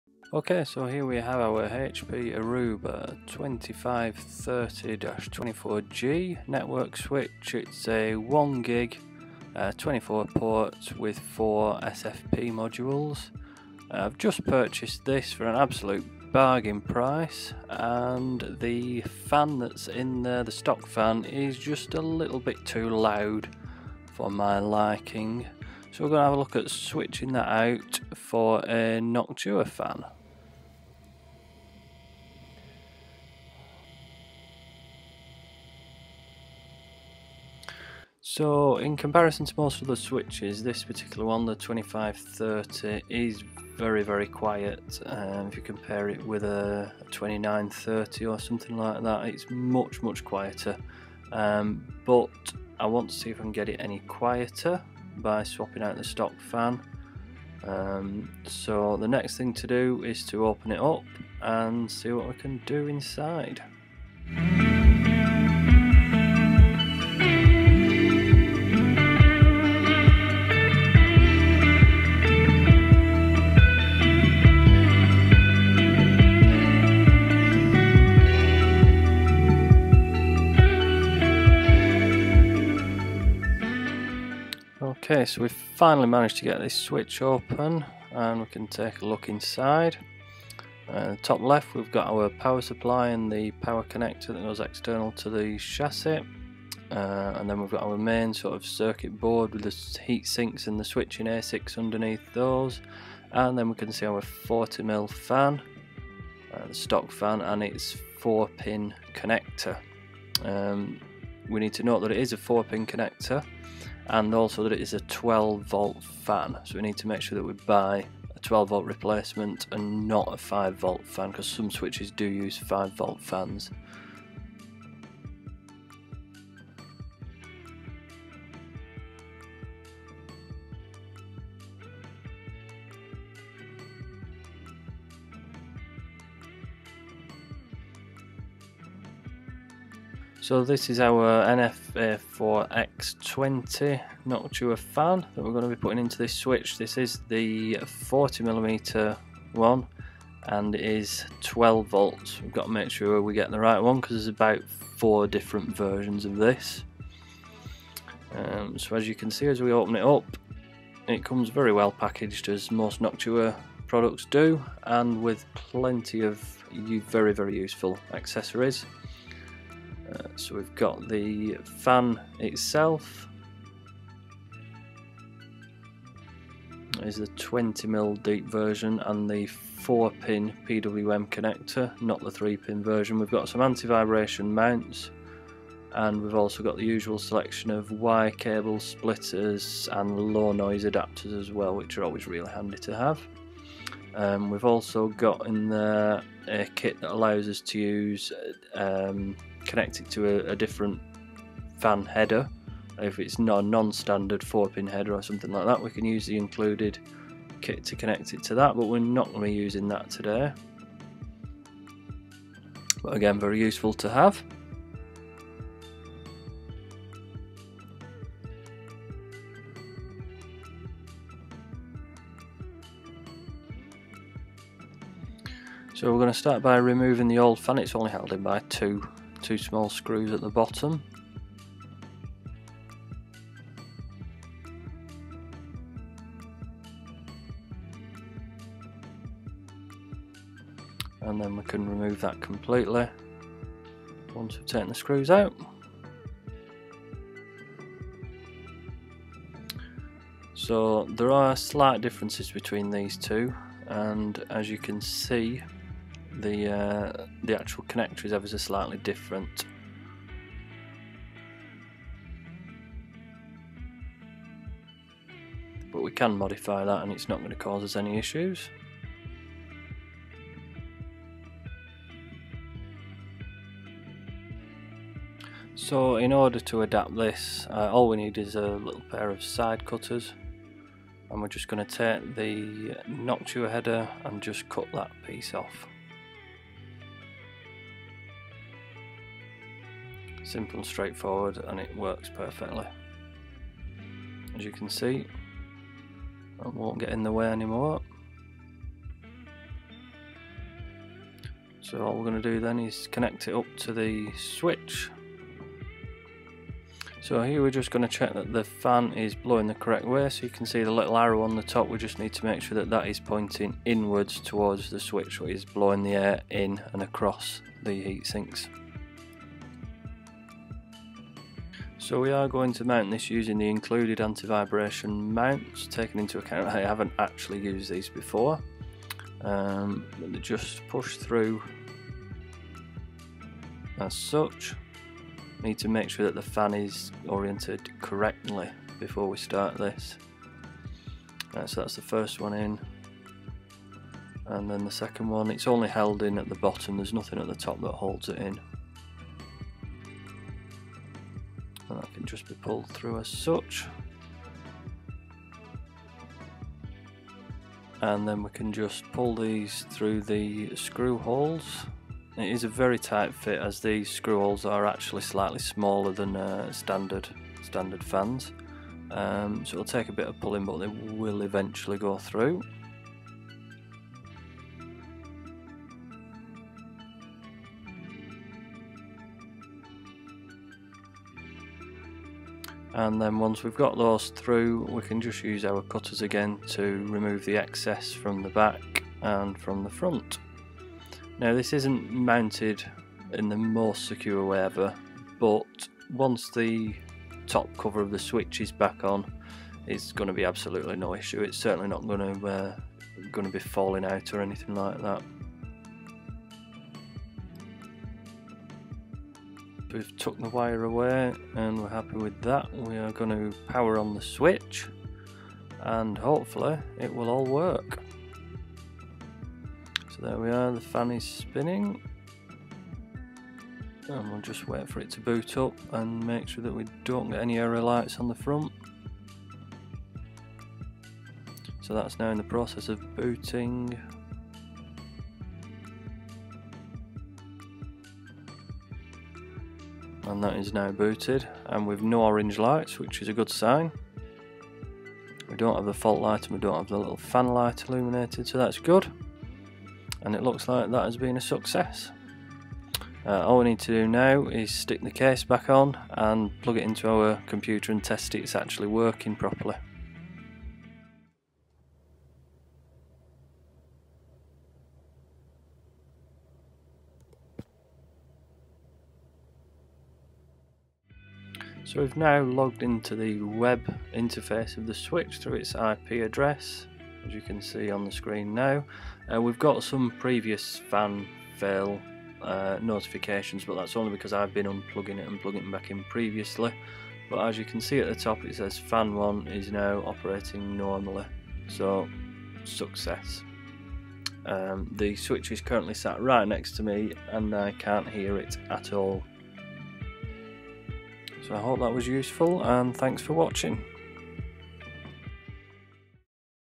HP Aruba 2530-24G J9776A Network Switch Noctua Fan Upgrade
Upgrading a HP/Aruba 2530-24G J9776A with a Noctua NF-A4x20 PWM Fan